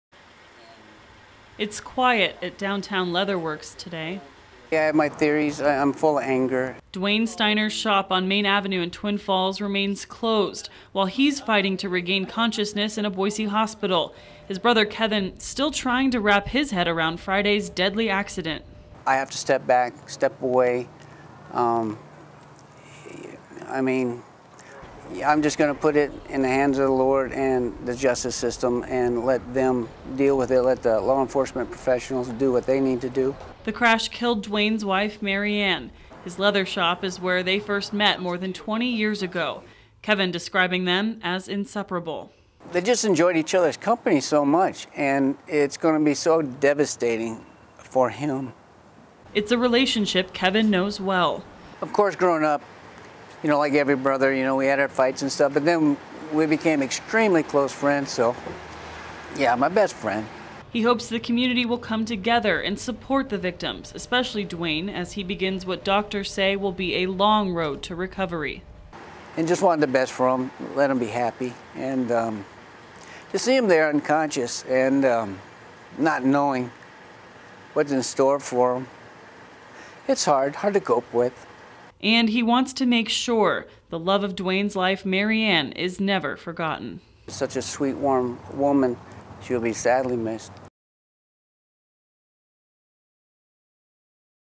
Crash Victim's Brother Speaks out
Kevin Steiner discusses his brother's recovery and tragedy